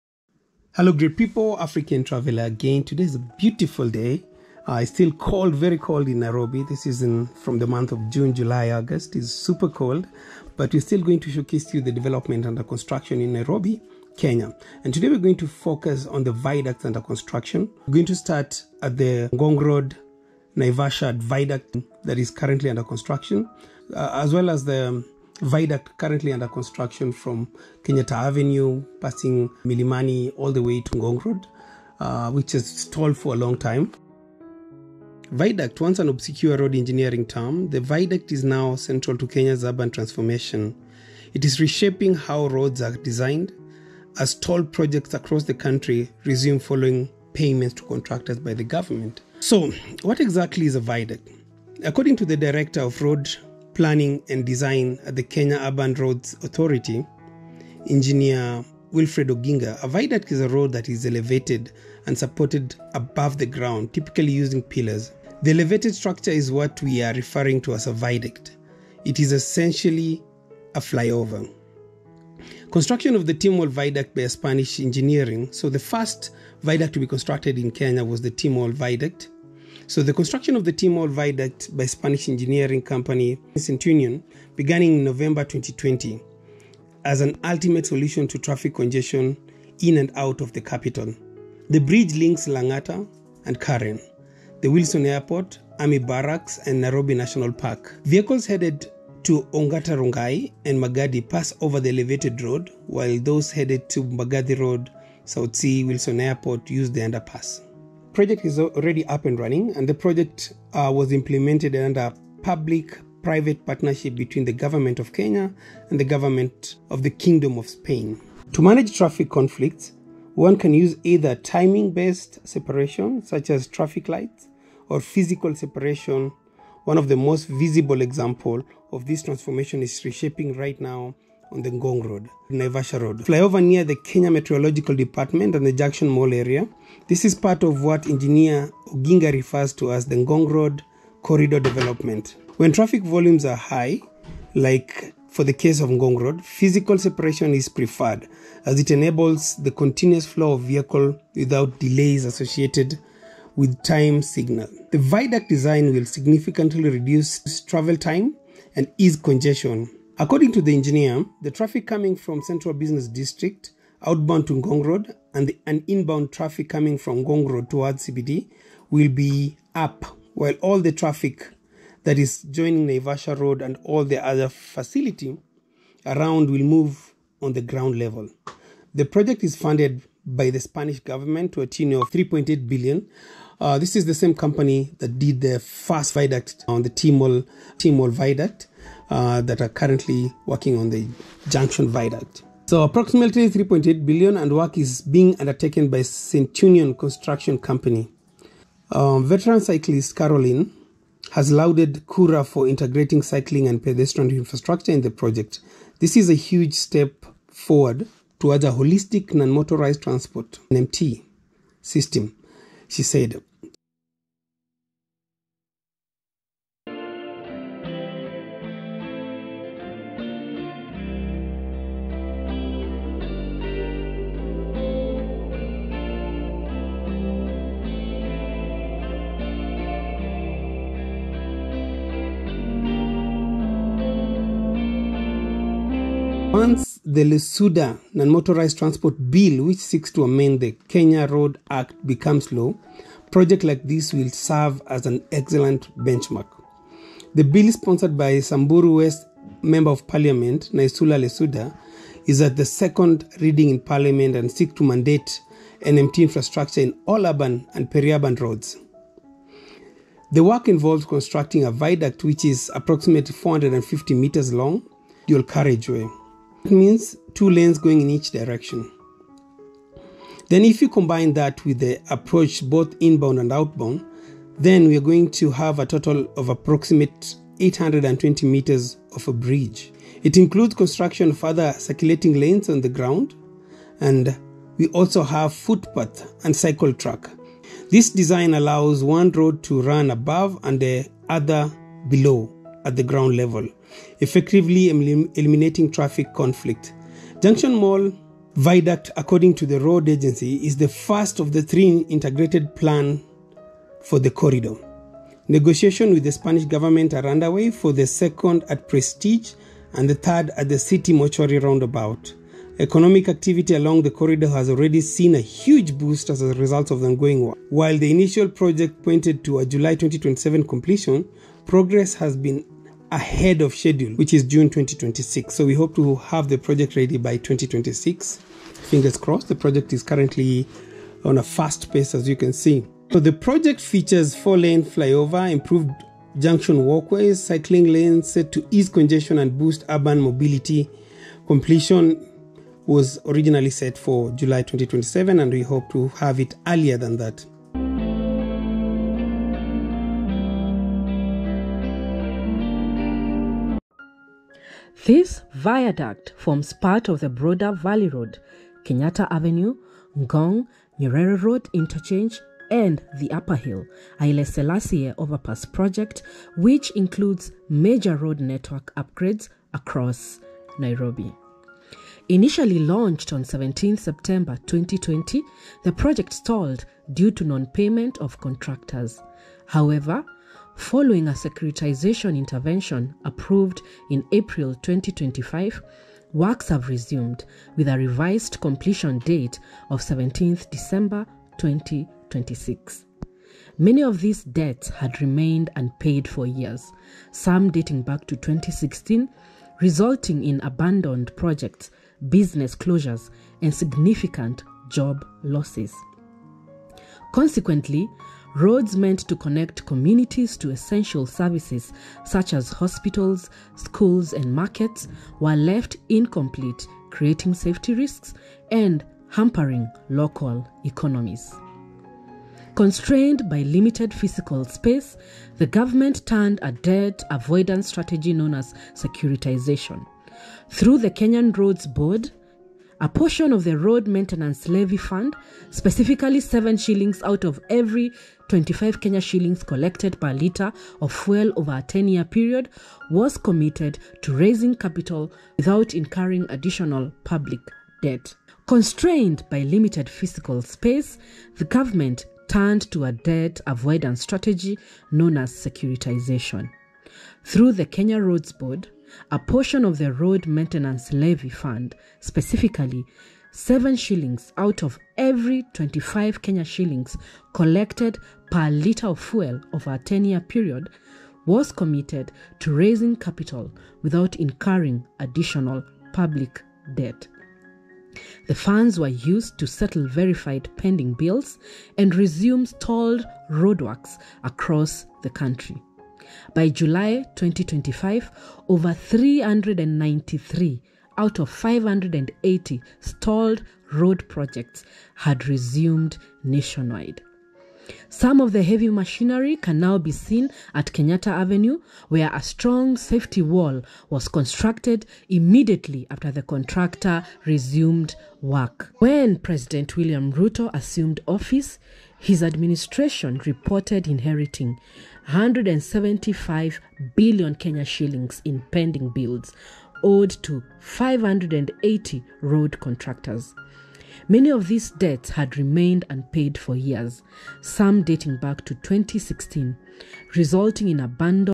Nairobi Junction Viaduct: Kenyatta Avenue Viaduct Transformation!"
Let's journey together!

Viaduct
Once an obscure road engineering term, the viaduct is now central to Kenya’s urban transformation. It is reshaping how roads are designed, as stalled projects across the country resume following payments to contractors.

So, what exactly is a viaduct?
According to the Director for Roads Planning and Design at  Kenya Urban Roads Authority,  Engineer Wilfred Oginga, a viaduct is a road that is elevated and supported above the ground, typically using pillars. The elevated structure is what we are referring to as a viaduct. it is essentially a flyover.

Construction of the T-Mall viaduct by Spanish engineering company CENTUNION began in November 2020 as an ultimate solution to traffic congestion in and out of the capital.

The bridge links Lang’ata and Karen, the Wilson airport, army barracks and Nairobi national park.

Vehicles headed to Ongata Rongai and Magadi pass over the elevated road while those headed to Mbagathi Road, South C, Wilson Airport  use the underpass.
 To manage traffic conflicts, one can use either timing-based separation, such as traffic lights or physical separationOne of the most visible examples of this transformation is happening right now on Ngong Road – Naivasha Road (Junction Mall) flyover particularly near the Kenya Meteorological Department and the Junction Mall area. This is part of what Engineer Oginga refers to as the Ngong Road Corridor Development.
“When traffic volumes are high like for the case of Ngong road, physical separation is preferred, as it enables a continuous flow of vehicles without the delays associated with timed signals. The viaduct design will significantly reduce travel time and ease congestion." He further elaborated.

According to the engineer, the traffic coming from the Central Business District (CBD) outbound to Ngong Town, and the inbound traffic coming from Ngong towards the CBD, will be up, while all the other traffic that is joining Naivasha Road and all the other facilities around will move on the ground level.

The project is funded by the Spanish government to the tune of 225 million Euros, approximately Ksh.3.8 billion and the work is being undertaken by Centunion Construction Company.

Veteran cyclist Caroline Mbutura has lauded KURA for integrating cycling and pedestrian infrastructure in the project.
“This is a huge step forward towards a holistic Non-Motorised Transport (NMT) system,” she said. “Once the Lesuuda Non-Motorised Transport Bill, which seeks to amend the Kenya Roads Act, becomes law, projects like this will serve as excellent benchmarks.”

“The work involves constructing a viaduct which is approximately 450 meters long, dual carriageway; that means two lanes going in each direction. Then, if you combine that with the approaches, both inbound and outbound, then we are going to have a total of approximately 820 metres of a bridge. It includes construction of other circulating lanes, on the ground, and we also have footpaths and cycle tracks.

“Negotiations with the Spanish government are underway for the second at Prestige, and the third at the City Mortuary Roundabout. Economic activity along the corridor has already seen a huge boost as a result of the ongoing works. The full project was originally scheduled for 36 months, plus a 12-month defects liability (maintenance) period. While initial projections pointed to a July 2027 completion, progress has been ahead of schedule;June 2026

Ngong Road – Naivasha Road Flyover Project
Construction of the Ngong Rd–Naivasha Rd (Junction Mall) Flyover is well underway! The project features a 4-lane flyover, improved junctions, walkways & cycling lanes, set to ease congestion and boost urban mobility. Completion is set for July 2027,"

The construction of the flyover comes at a time when the authority has ramped up efforts to construct and rehabilitate the city's transport infrastructure.

How pillar-filled project along Kenyatta Avenue will look like after completion
Once completed, the project will allow motorists travelling from the Central Business District to connect directly to Ngong Road near the Kenya National Library Service.Nairobi residents using Kenyatta Avenue,Valley Road and State House Road routes have taken notice of a set of large concrete pillars recently installed along the centre of Kenyatta Avenue.